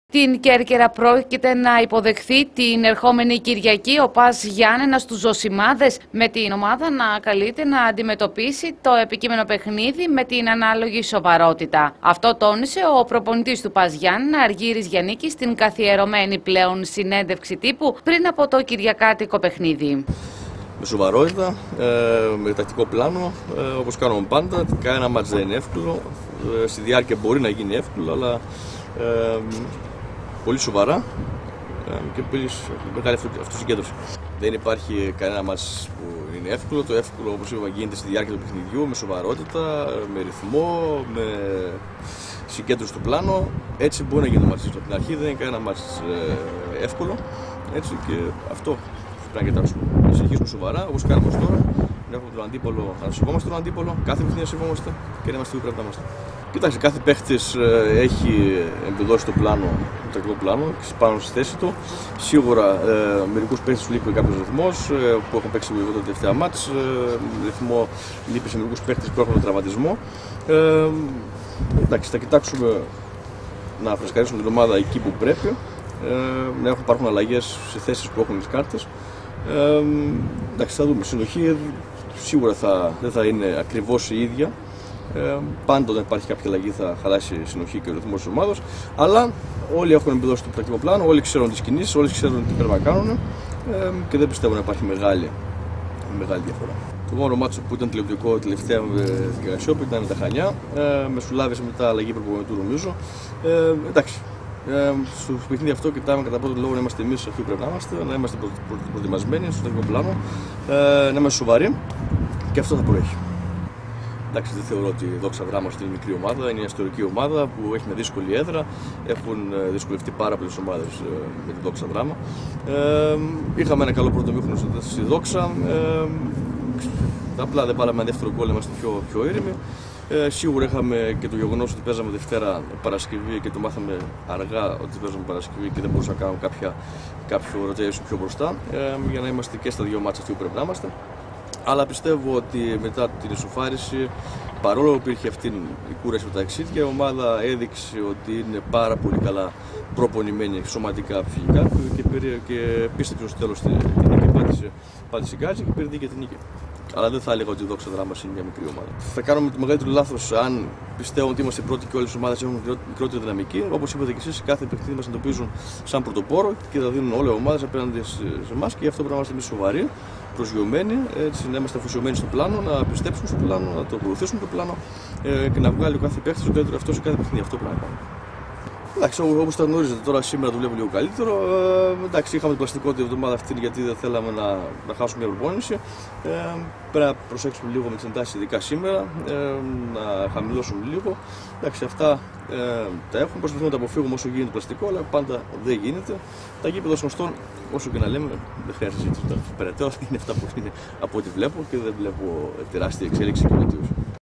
Δηλώσεις Γιαννίκη για το παιχνίδι της Κυριακής με την Κέρκυρα
| Δηλώσεις Γιαννίκη για το παιχνίδι της Κυριακής με την Κέρκυρα | 06/03/2020